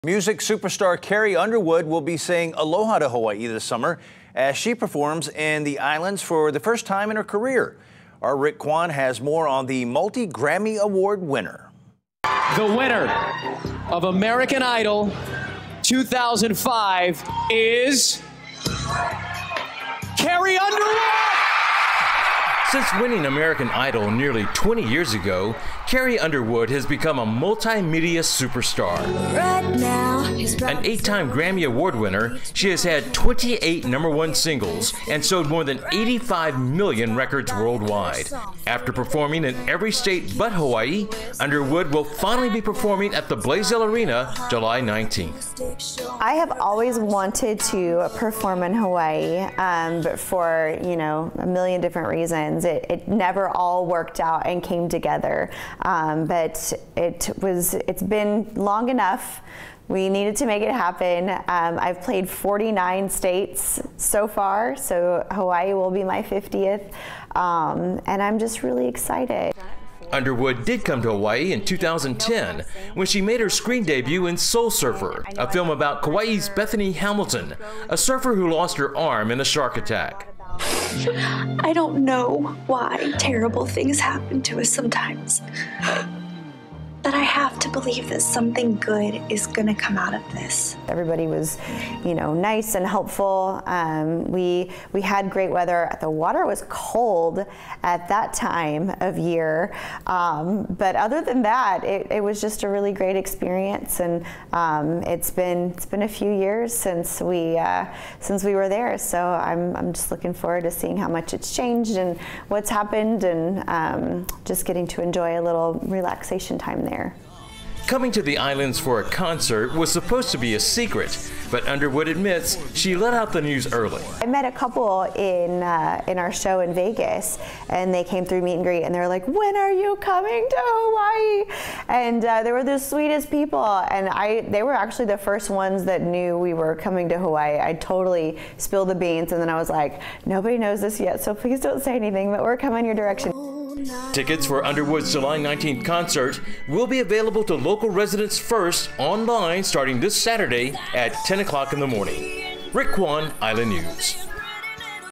Country music superstar Carrie Underwood to rock Honolulu's Blaisdell Arena
Music superstar Carrie Underwood will be saying "Aloha" to Hawaii this summer, as she performs in the islands for the first time in her career.